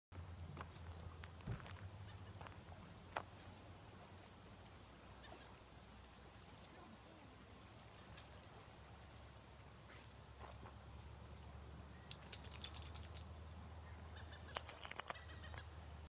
Beautiful Yellowknife River, Canada
The Yellowknife River is beautiful. The water is so blue, so clear.
This past weekend we spotted a Snowy Owl right here.